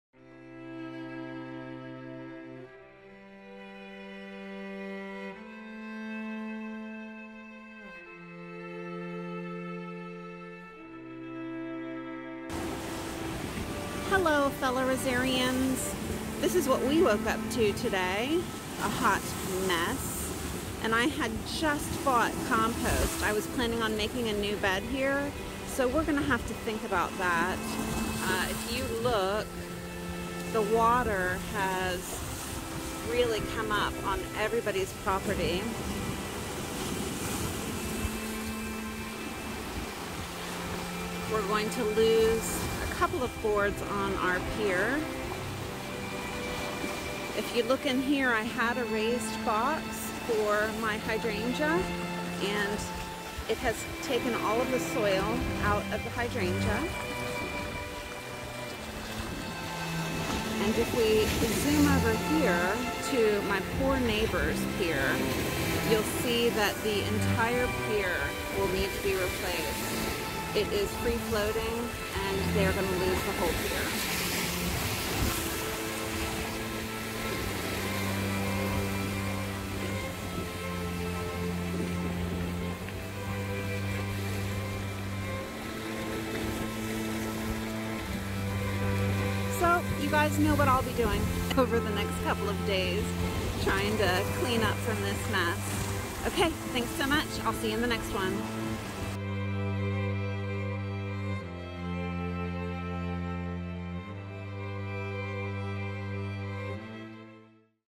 🌹 Coastal Flood Impact on Waterfront Gardens // Brackish Water
Today the Chesapeake Bay area experienced the largest storm surge and coastal flooding since 2003.  I share before pics of our waterfront gardens and discuss the impact of the brackish coastal flooding on our beds and plants along the river.

Please share in the comments if your gardens have been impacted by severe weather. If you join our FB group, Rose Geeks, you can share pictures with us.

🌹 SOCIAL MEDIA 🌹
🌎 Rose Geeks Facebook Group to Share Your Roses